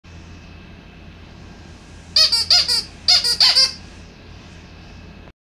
Minnie Mouse Bow-tique Honker Horn
Whenever someone isn't paying attention when I call out "Passing on your left!" a quick couple of squeezes will get their attention every time.  It also drives dogs crazy, an added bonus.